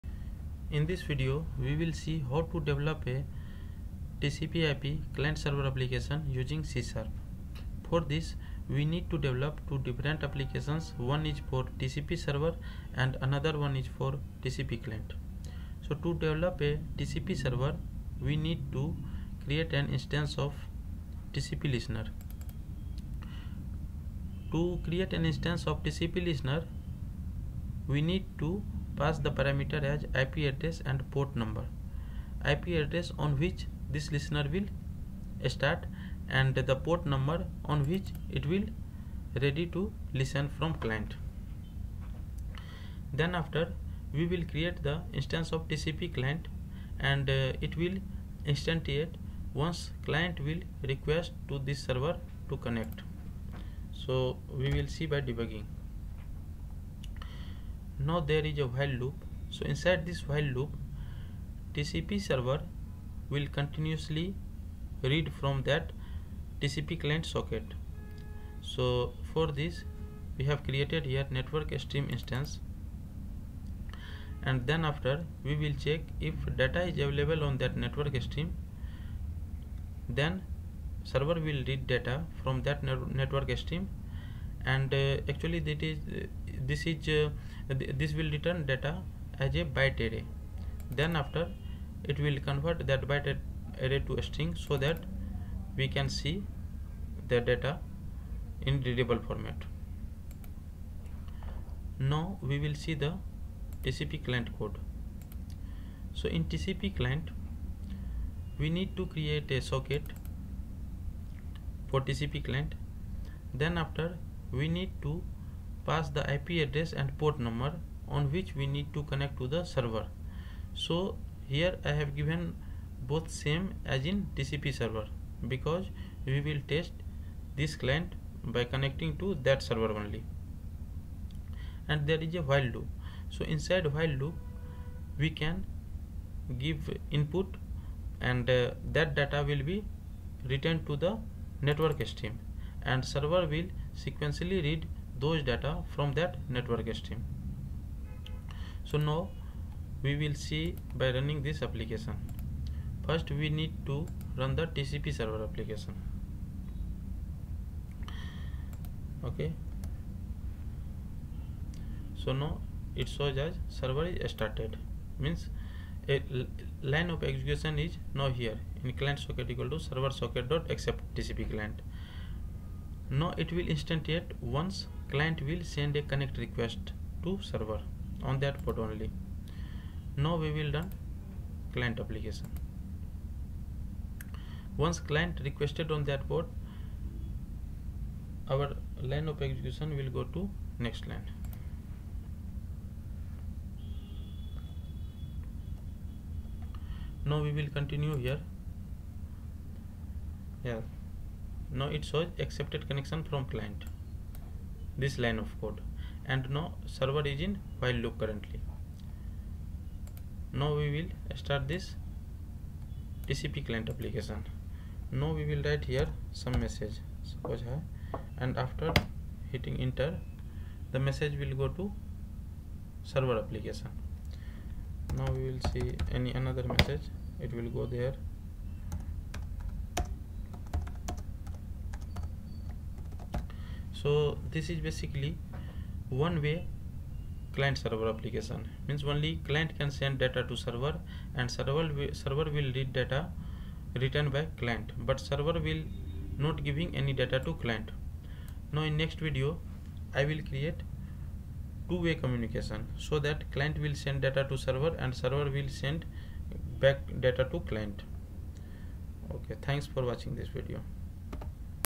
TCP/IP Client Server Application Using C#
Client Application can send data to server application using TCP connection